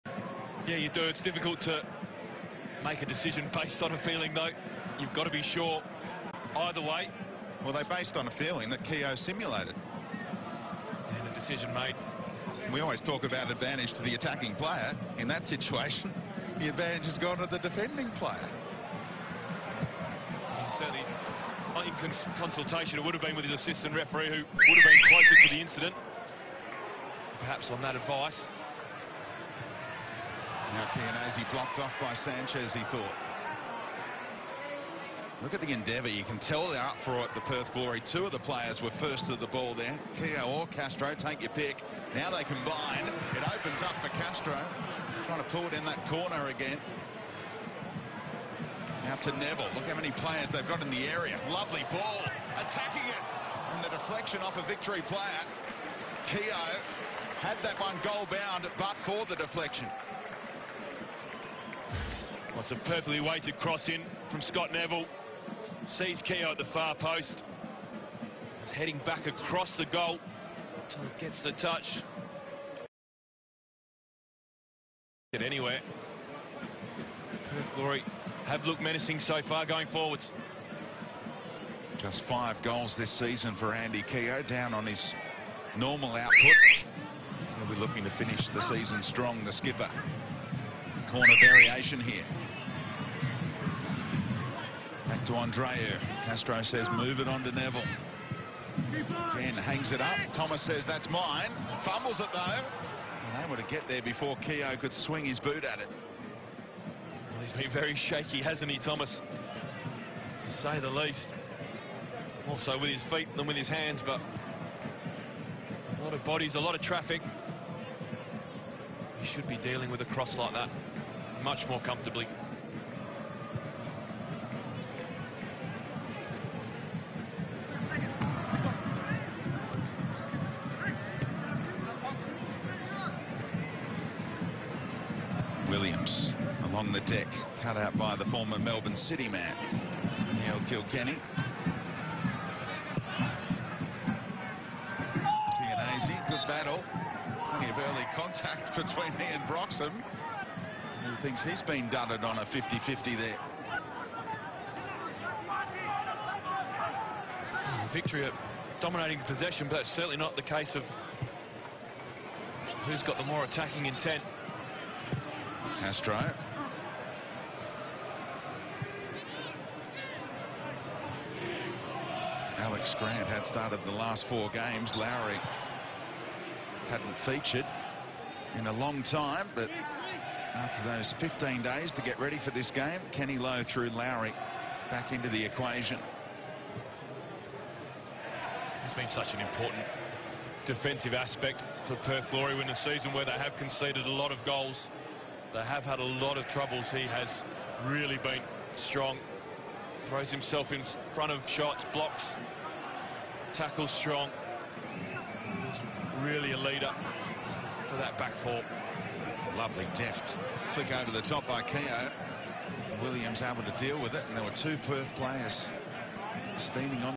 Perth Glory V Melbourne Victory đang thi đấu, 2018
1. Trong cuộc đối đầu gần nhất, Melbourne Victory thắng với tỷ số 3-2.
2. Ở 4 cuộc đụng độ giữa 2 đội, Perth Glory thắng 1 và thua 3.
3. 4 trận gần nhất, Melbourne Victory giành tới 3 chiến thắng, còn lại 1 trận hòa.